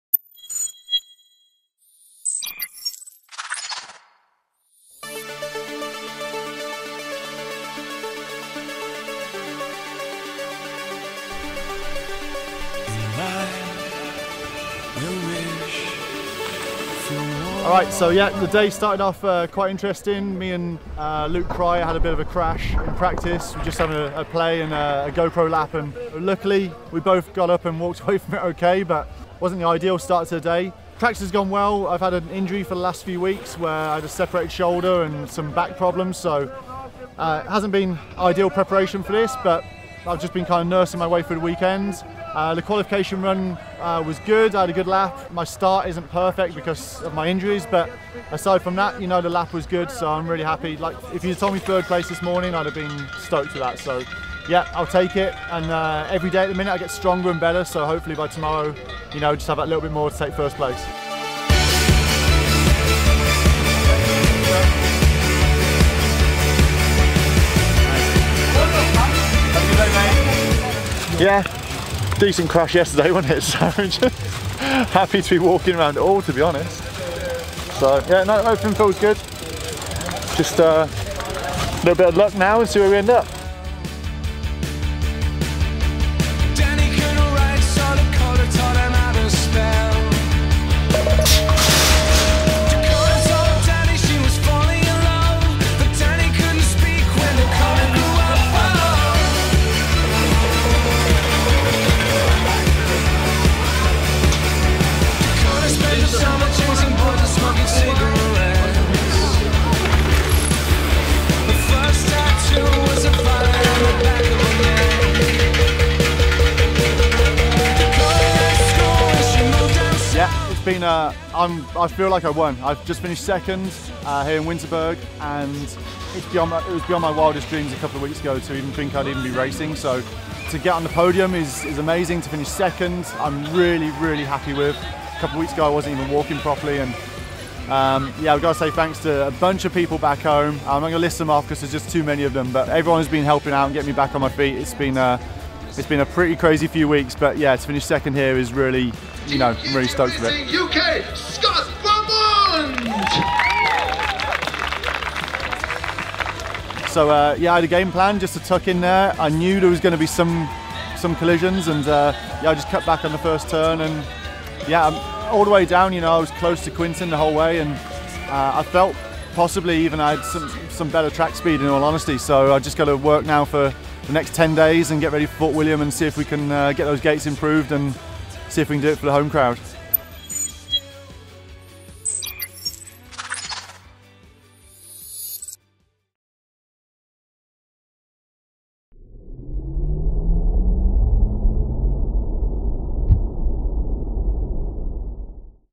Yeti Racing UK 2015 - Podcast 1 - Winterberg, Germany
Round 1 of the 2015 4X ProTour was held in Winterberg, Germany during the huge DirtMasters festival.

Scott Beaumont, fresh back from 10 weeks of injury went to round 1 with the hope of salvaging some points towards the 2015 4X ProTour overall title. Things went a bit better than that though.